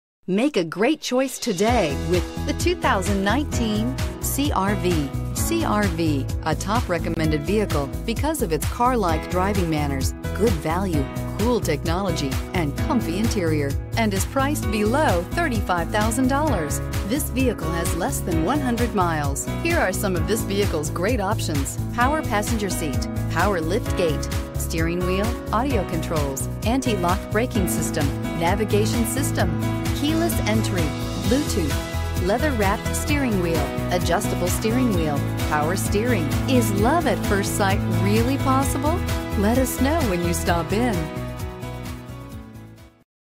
2019 Honda Cr-V 38022
2019 HONDA CR-V Mesa, AZ
Stock# 38022

6229 E Auto Park Dr

You'll love this 2019 Honda Cr-V. This is a car you'll want to take home. With 8 miles, it features Variable transmission and an exterior color of GUNMETAL METALL. Call us and be the first to open the car door today!